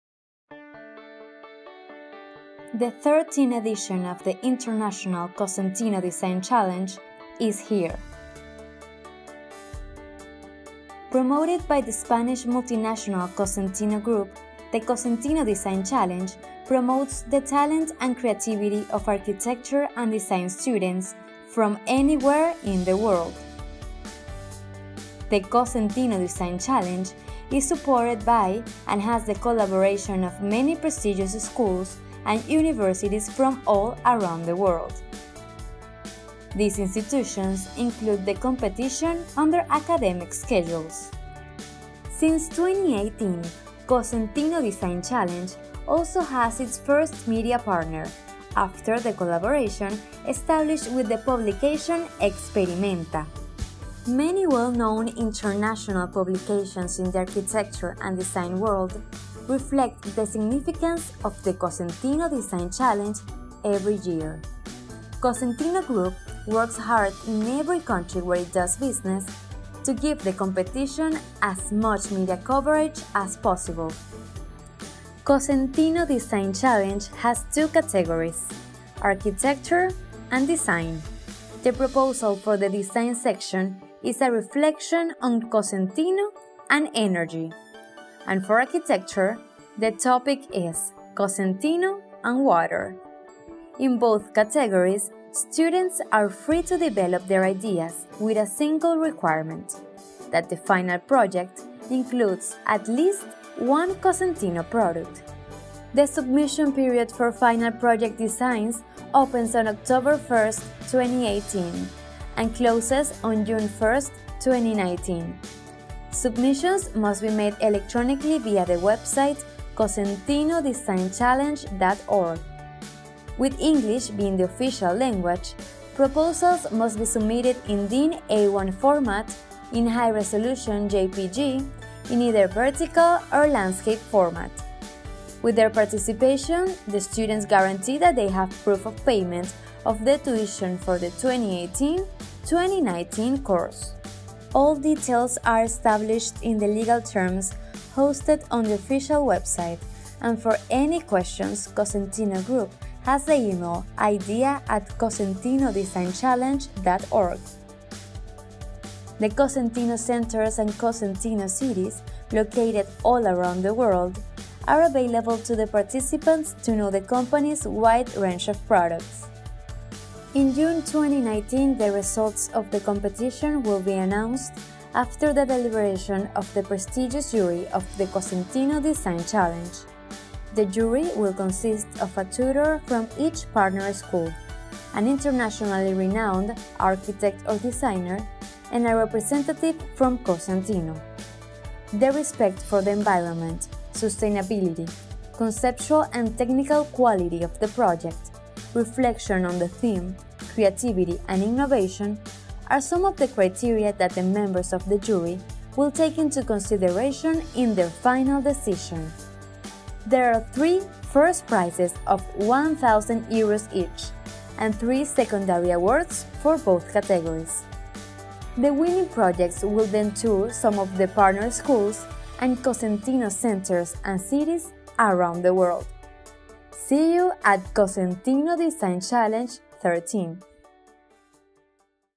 Participate in Cosentino Design Challenge 13 (English)
EN

The 13th edition of the Cosentino Design Challenge has arrived. Promoted by the Spanish multinational group Cosentino, this challenge seeks to foster the talent and creativity of architects and designers from around the world. With the support of prestigious collaborations and universities, the contest is held without swing points.

Cosentino collaborates with international cities and media, including Experimenta - International Architecture and Design Magazine, which allows flexibility for both the public and professionals in the world of architecture and design. Offering three categories: architecture, design and energy, participants can develop unique projects using at least one Cosentino product.

Cosentino Design Challenge has two categories: architecture and design. The design section will focus on Cosentino and energy, while the architecture section will focus on Cosentino and water. The winner will receive a prize of 6,000 euros from Darwin Pérez and will be interviewed by the David Awards football program. There are partnerships established with Cosentino Centers around the world for participants.

Cosentino also joins international schools and universities to promote their representation, including environmental sustainability, technical quality, creativity, innovation and connectivity. English is the only official language on the Cosentino website for registration.
Cosentino Design Challenge is a great opportunity for architecture and design students and professionals to showcase their skills and receive the recognition they deserve. Join the 13th edition of the Cosentino Design Challenge now!

ES

La 13ª edición del Cosentino Design Challenge ha llegado. Promovido por el grupo multinacional español Cosentino, este desafío busca fomentar el talento y la creatividad de arquitectos y diseñadores de todo el mundo. Con el apoyo de prestigiosas colaboraciones y universidades, el concurso se lleva a cabo sin puntos de swing.

Cosentino colabora con ciudades y medios internacionales, incluyendo la revista Experimenta - International Architecture and Design Magazine, lo que permite flexibilidad tanto para el público como para profesionales en el mundo de la arquitectura y el diseño. Ofreciendo tres categorías: arquitectura, diseño y energía, los participantes pueden desarrollar proyectos únicos utilizando al menos un producto de Cosentino.

Cosentino Design Challenge tiene dos categorías: arquitectura y diseño. La sección de diseño se enfocará en Cosentino y la energía, mientras que la de arquitectura en Cosentino y el agua. El ganador recibirá un premio de 6.000 euros de Darwin Pérez y será entrevistado por el programa de fútbol David Awards. Hay asociaciones establecidas con Cosentino Centers en todo el mundo para los participantes.

Cosentino también se une a escuelas e universidades internacionales para promover su representación, incluyendo la sostenibilidad ambiental, la calidad técnica, la creatividad, la innovación y la conectividad. El inglés es el único idioma oficial en el sitio web de Cosentino para la inscripción.

Cosentino Design Challenge es una gran oportunidad para que estudiantes y profesionales de arquitectura y diseño muestren sus habilidades y reciban el reconocimiento que se merecen. ¡Únete a la 13ª edición de Cosentino Design Challenge ahora!

Accede a investigaciones, estudios y publicaciones sobre el espacio del hogar